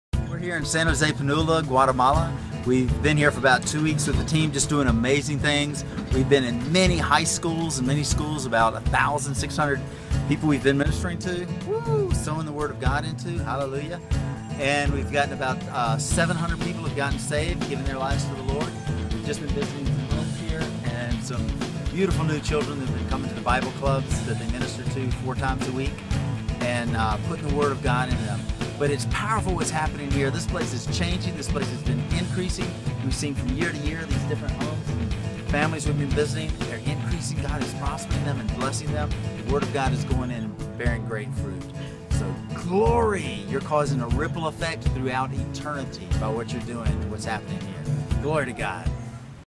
Quick Guate Missions Update June2015
Brief update of Guatemala Missions Team Trip June 2015 in San Jose Pinula. We are as we Share the Love, Power & Blessings of God to the ends of the earth! Gloory to God